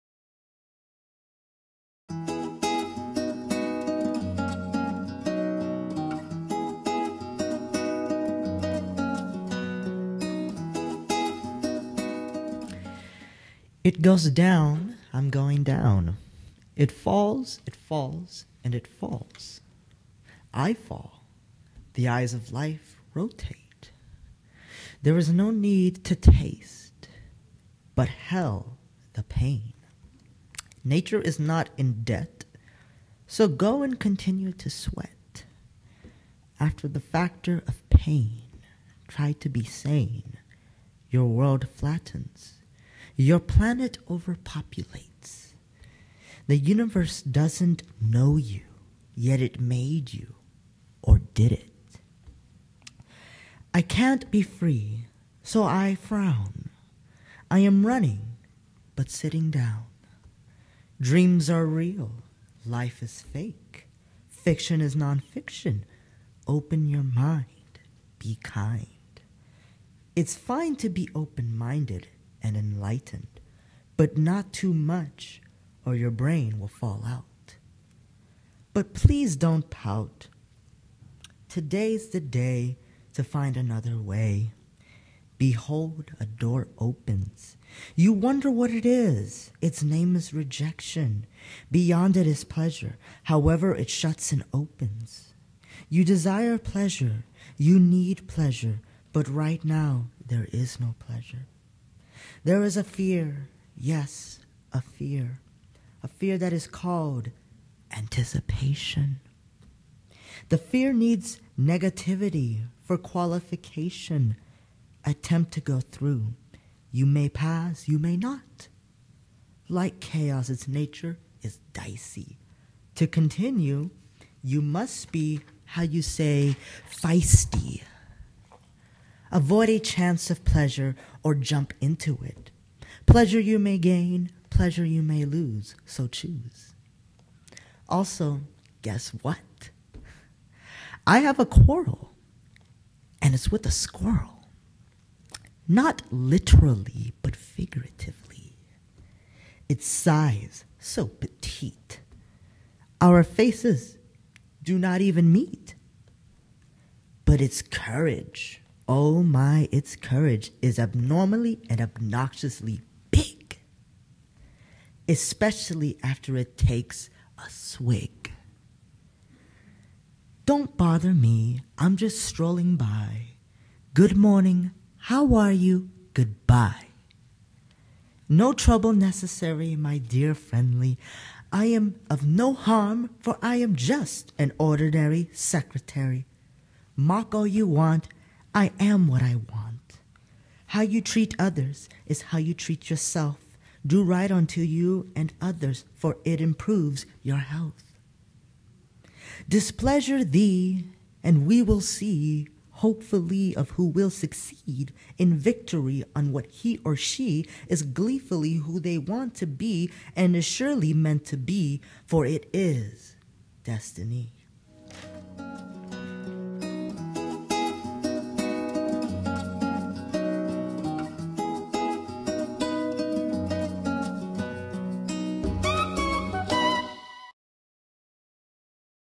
Norco College Poetry Performance - Chris Martin
The Wilfred J. Airey Library at Norco College - Poetry Performance on November 5, 2015 featuring Chris Martin reciting his poem "Untitled Life."